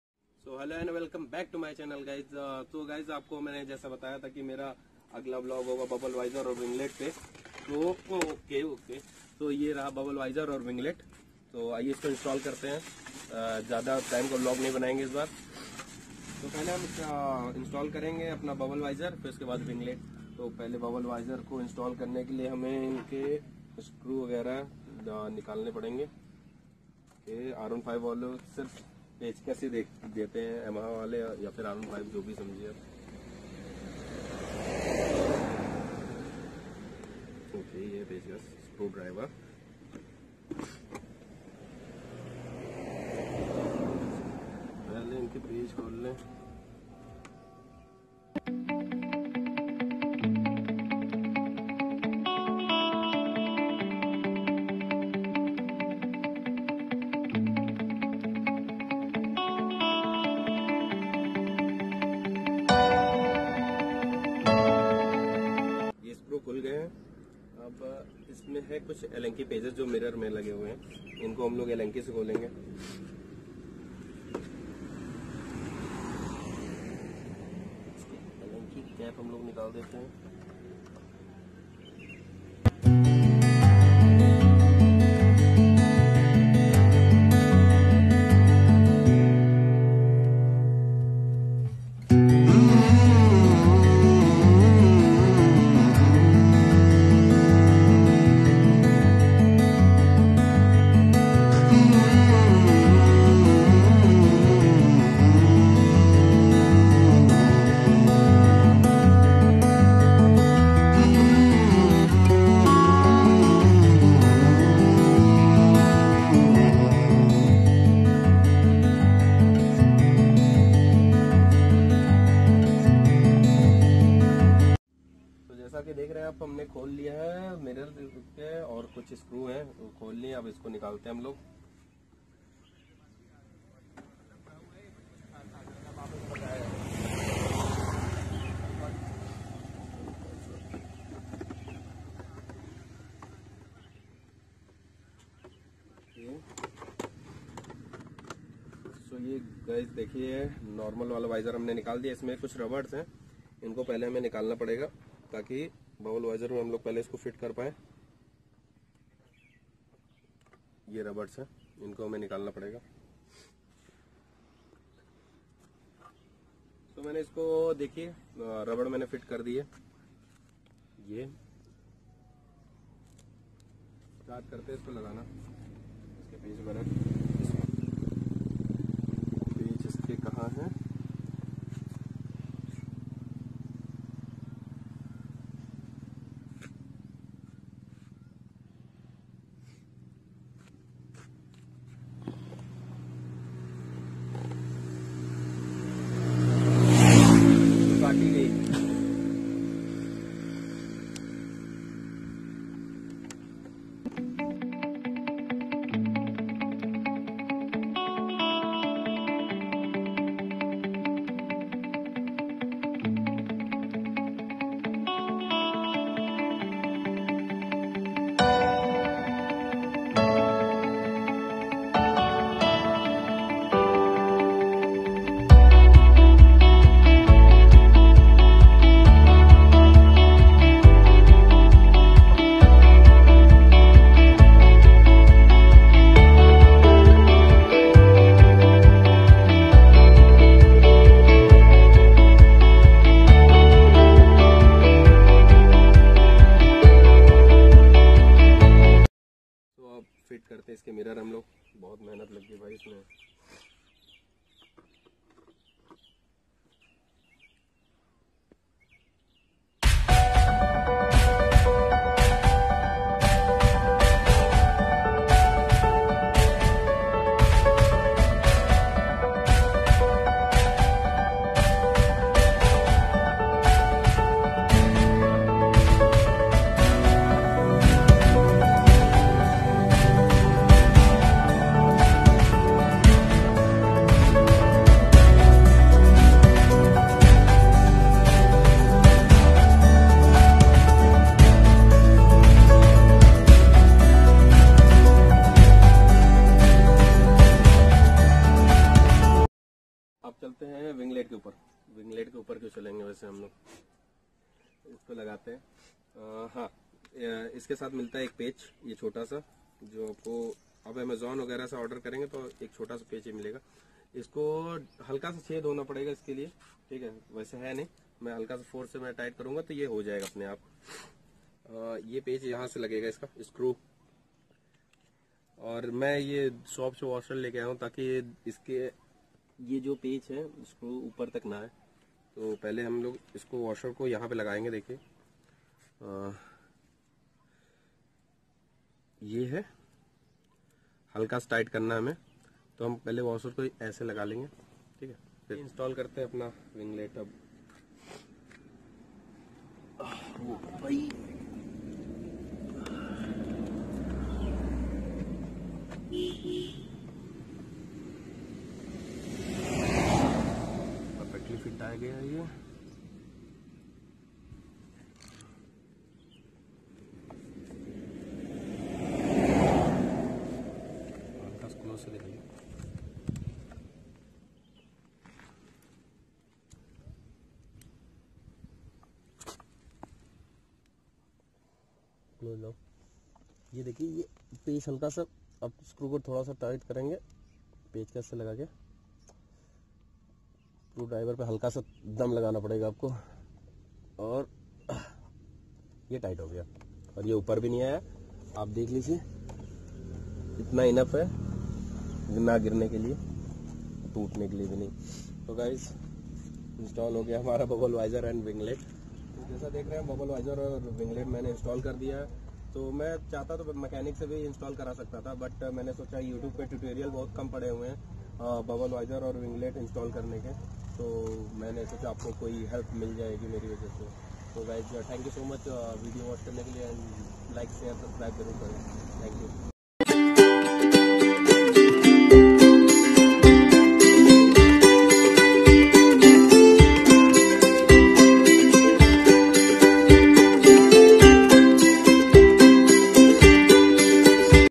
INSTALLING || BUBBLE VISOR AND WINGLET || TUTORIAL || R15 V3
BUBBLE VISOR

bubble Visor, specially designed for Yamaha R15 V3 model will be your perfect choice. double bubble visor can ensure you double the functionality than a normal windscreen by blocking more wind than usual shielding. Being one of the popular options for comfort riding, you will be availed with a lasting design along with your safe ride. Enjoy your ride with the cold wind with protection against bugs and frost

WINGLET

Custom winglet version 1 designed and manufactured for the all new Yamaha Version 3.0. This winglet is made out of Polypropylene and is the same material of the motorcycle's fairing. This visor will give the motorcycle a unique aggressive and sporty look.

Custom designed winglet
Made with polypropylene material
Direct fit product
Provides better aggressive and racing inspired look
Material: Fiber

LINK-BUBBLE VISOR